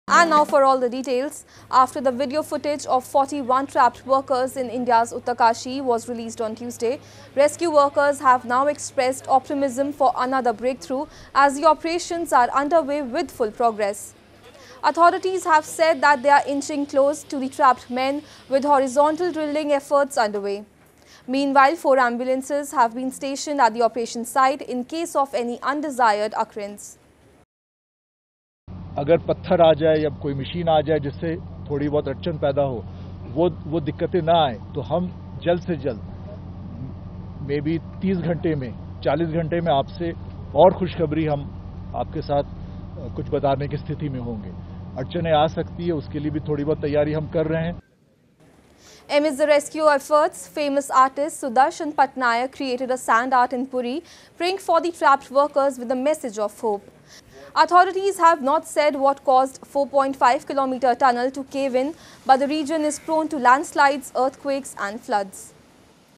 Rescuers expect another breakthrough for 41 trapped workers in India’s Uttarkashi
After the video footage of 41 trapped workers in India’s Uttarkashi was released on Tuesday, rescue workers have now expressed optimism for another breakthrough as the operations are underway with full progress. Authorities have said that they are inching closer to the trapped men, with horizontal drilling efforts underway. Meanwhile, four ambulances have been stationed at the operation site in case of any undesired occurrence.